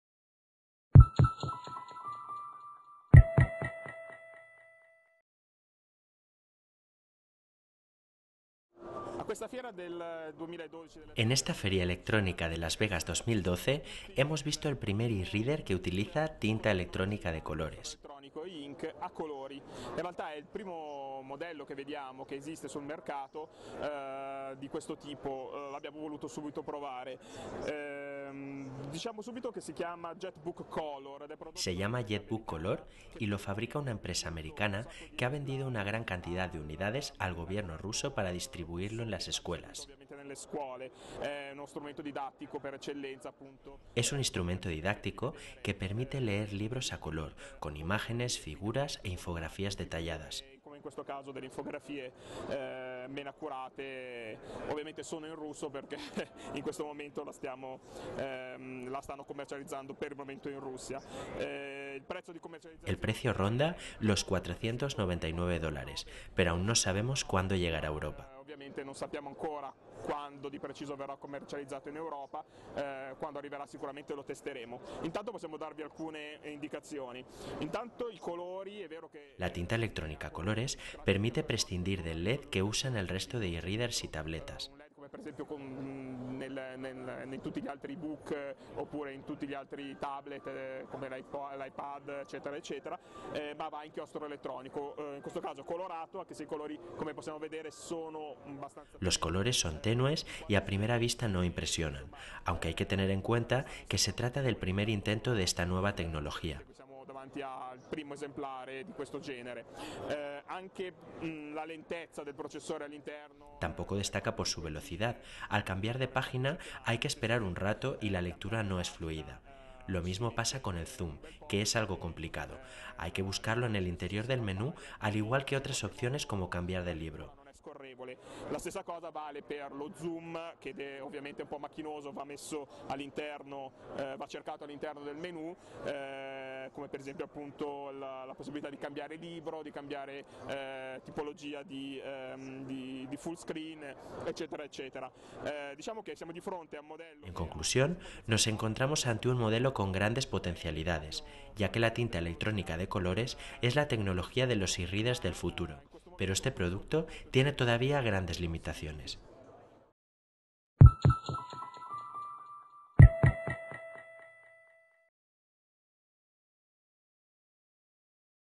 Jetbook, un eReader en color
En esta feria electrónica de Las Vegas 2012 hemos visto el primer eReader que utiliza tinta electrónica de colores. Se llama JetBook Color y lo fabrica una empresa americana que ha vendido una gran cantidad de unidades al Gobierno ruso para distribuirlo en las escuelas. Es un instrumento didáctico que permite leer libros a color, con imágenes, figuras e infografías detalladas. El precio ronda los 499 dólares, pero aún no sabemos cuándo llegará a Europa. La tinta electrónica a colores permite prescindir del LED que usan el resto de eReaders y tabletas. Los colores son tenues y a primera vista no impresionan, aunque hay que tener en cuenta que se trata el primer intento de esta nueva tecnología. Tampoco destaca por su velocidad: al cambiar página hay que esperar un rato y la lectura no es fluida. Lo mismo vale para el zoom, que es algo complicado: hay que buscarlo en el interior del menú al igual que otras opciones como cambiar de libro. En conclusión, nos encontramos ante un modelo con grandes potencialidades, ya que la tinta electrónica de colores es la tecnología de los eReaders del futuro. Pero este producto cuenta todavía con grandes limitaciones.